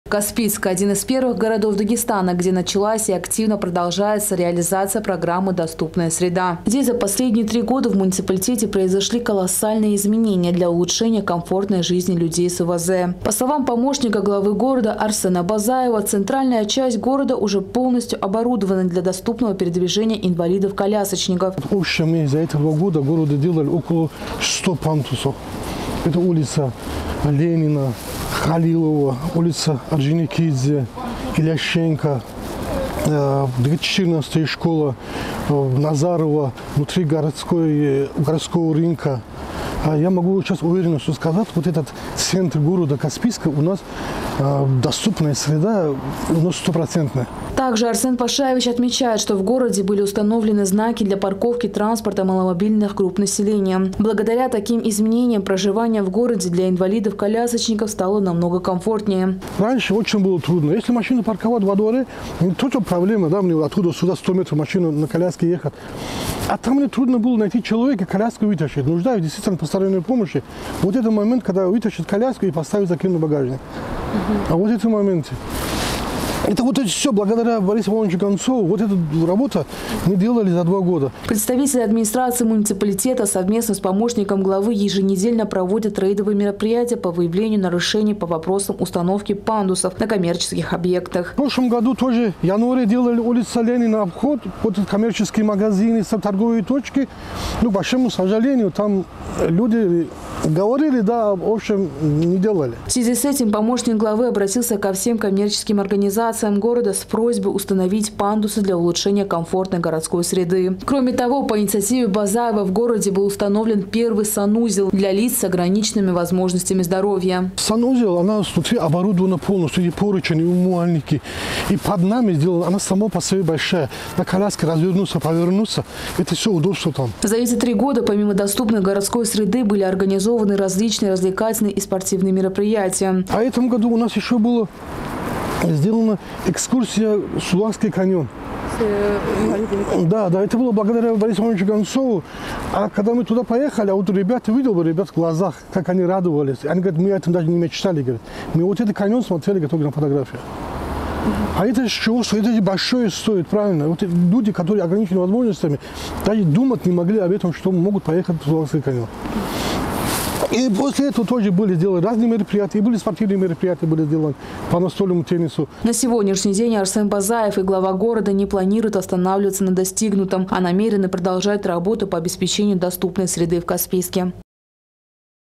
Каспийчане с ОВЗ живут полноценной и интересной жизнью благодаря доступной городской среде
Каспийск - один из первых городов Дагестана, где началась и активно продолжается реализация программы доступная среда. За последние три года в муниципалитете произошли колоссальные изменения для улучшения комфортной жизни людей с ОВЗ. По словам помощника главы города Арсена Базаева, центральная часть города уже полностью оборудована для доступного передвижения инвалидов-колясочников.

Также Арсен Пашаевич отмечает, что в городе были установлены знаки для парковки транспорта маломобильных групп населения. Благодаря таким изменением, проживание в городе для инвалидов колясничков стало намного комфортнее.